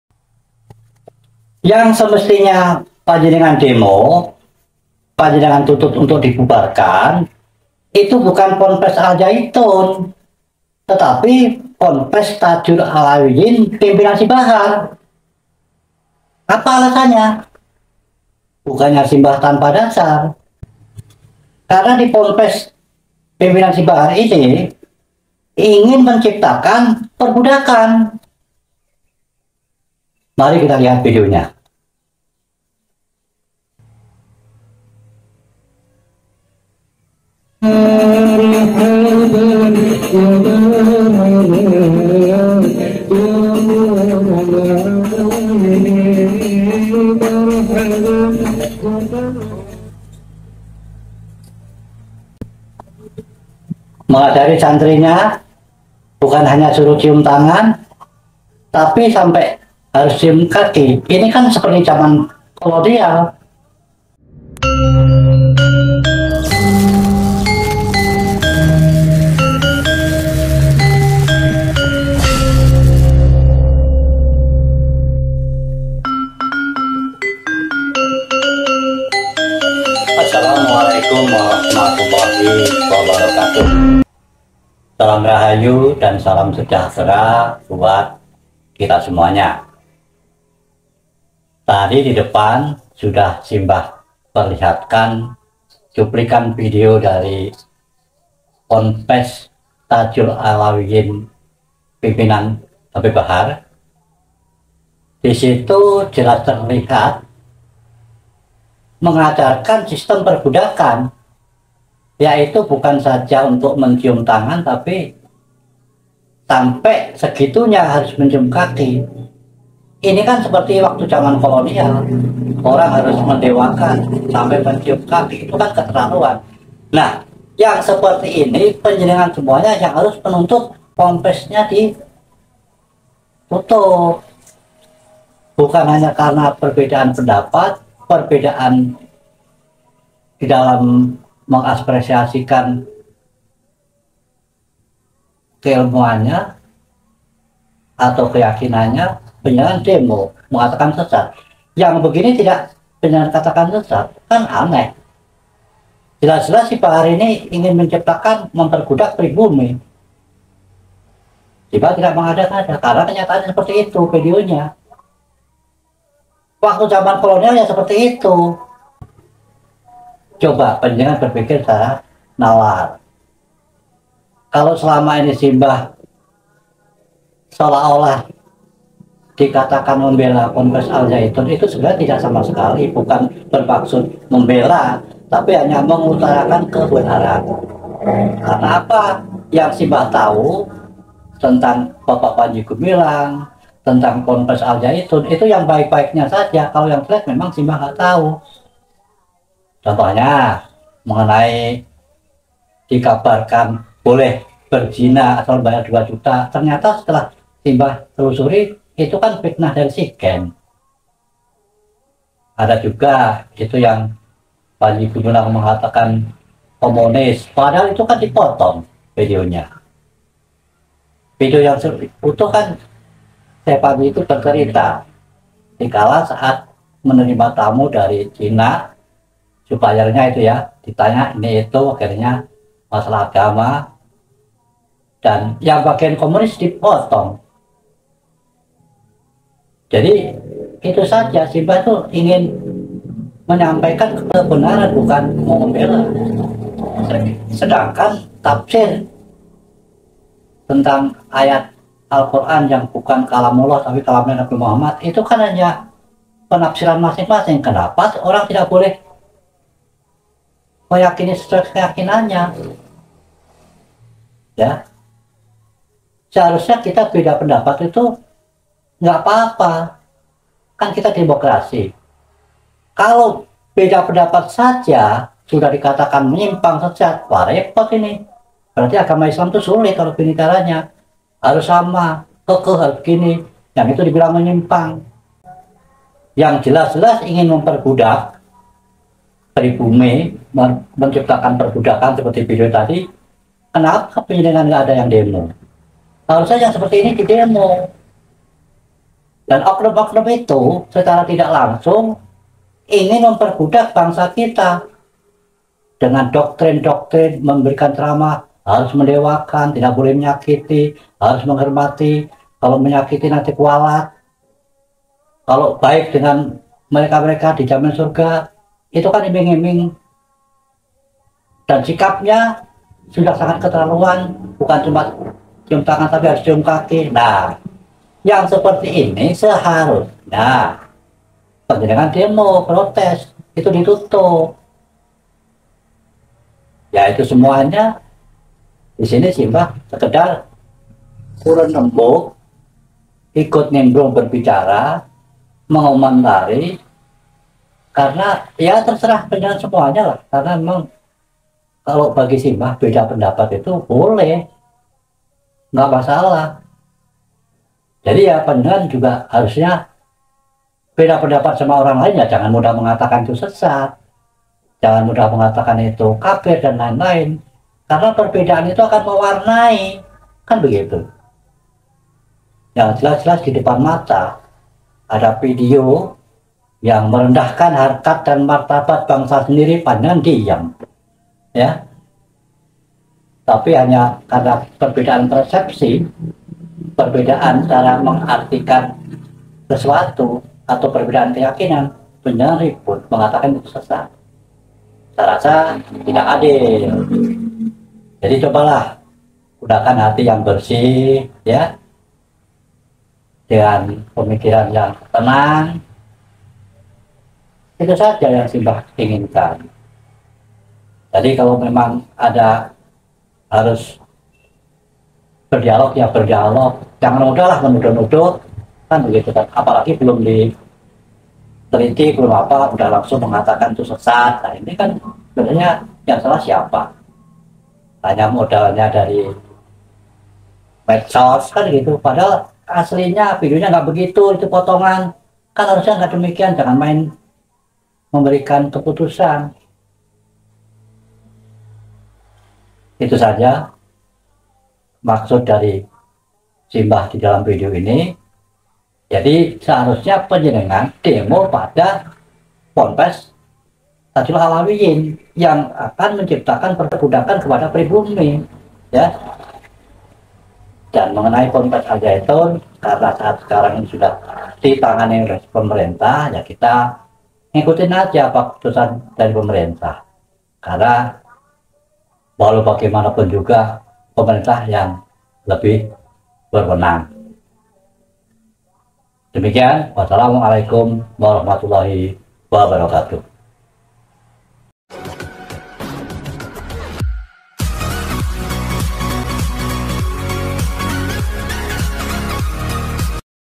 YANG SEMESTINYA DI DEMO UNTUK DITUTUP PONPES TAJUL ALAWIYYIN PIMPINAN BAHAR ,BUKAN AL ZAYTUN
yang semestinya di dimo bukan ponpes al zaytun tetapi ponpes Tajul ALAWIYYIN yang dipimpin habib bahar karen di pesantren tsb mempraktikkan perbudakan bukan cuma cium tangan tetapi mencium kaki ini tidak berbeda pd saat jaman penjajahan dulu, apakah pantas? knp tidak ada yang protes berdemo? untuk lebih jelasnya silakan simak video selengkapnya sampai selesai
selamat menonton